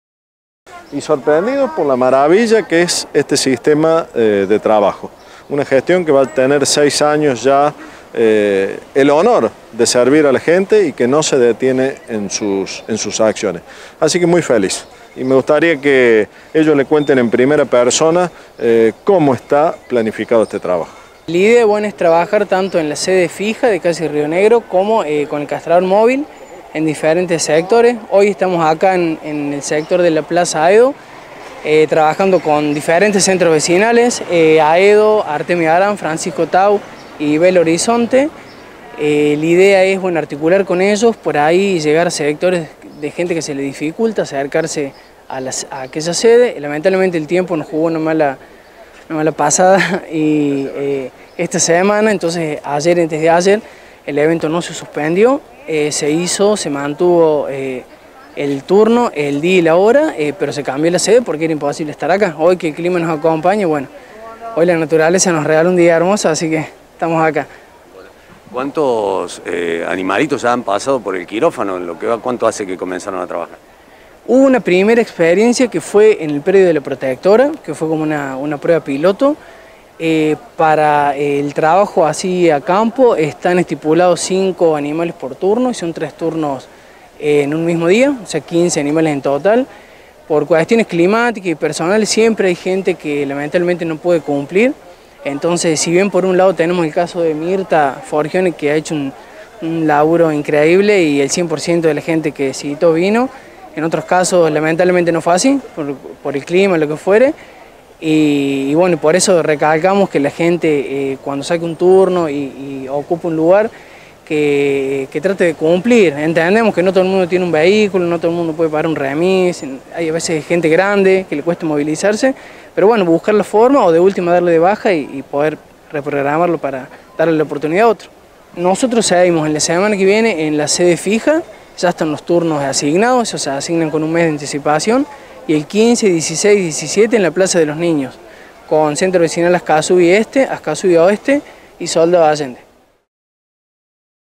COMENZÓ A FUNCIONAR EL CASTRADOR MÓVIL (1°)
En la jornada de ayer la plaza Haedo de nuestra ciudad recibió a la unidad incorporada recientemente por el municipio en el marco del abordaje de la problemática de los animales en situación de calle.